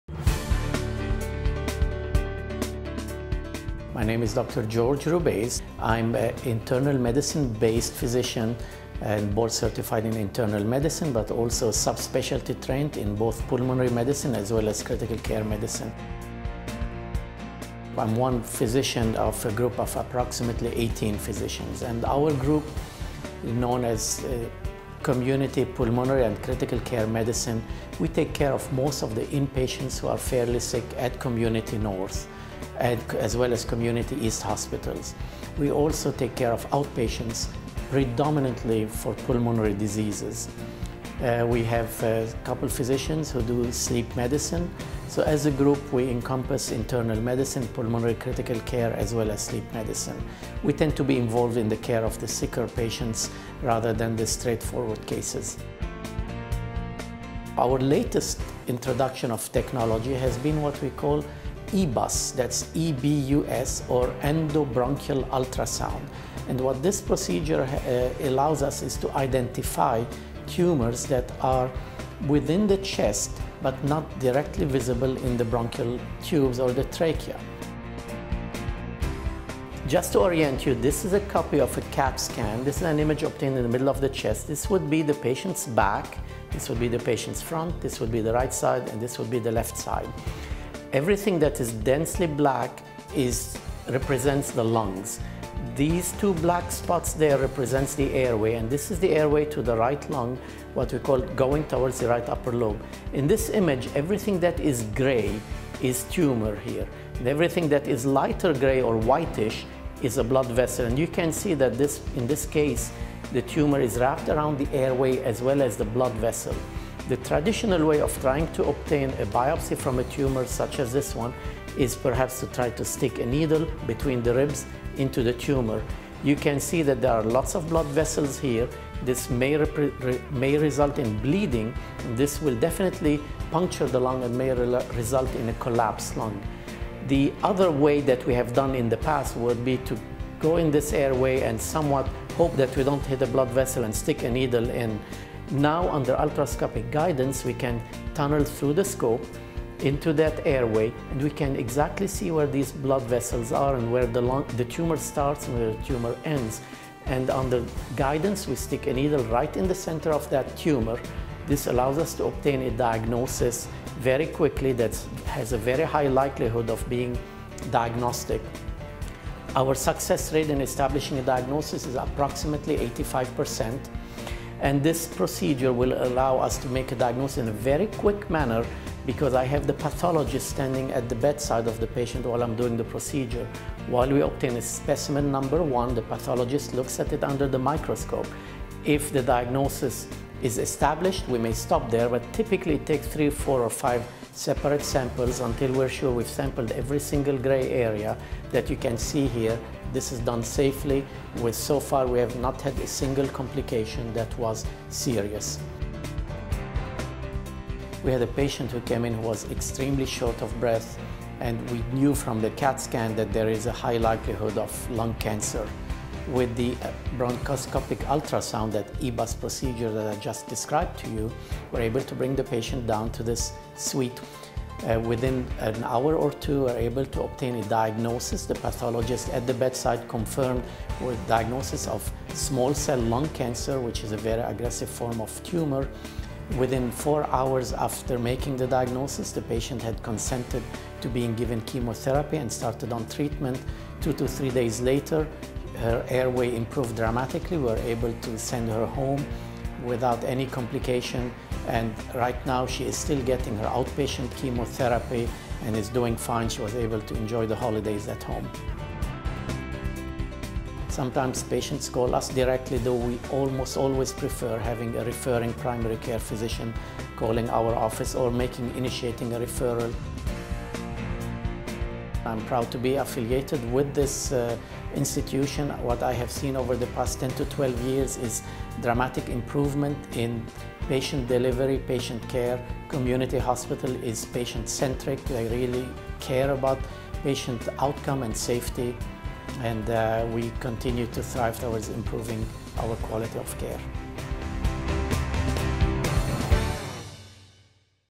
Meet Dr. George Rubeiz - Critical Care Medicine, Pulmonary Disease Medicine
Dr. George Rubeiz, pulmonologist, demonstrates the effectiveness and benefits of detecting and diagnosing cancer in the chest using the EBUS (Endobronchial Ultrasound) procedure.